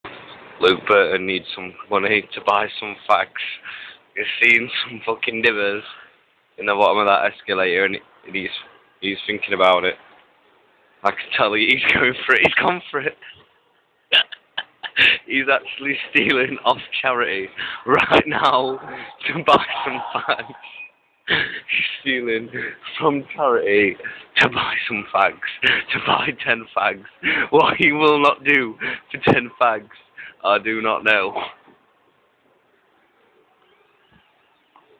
Meadowhall Mischief
stealing money from the meadowhall escalater fountain thing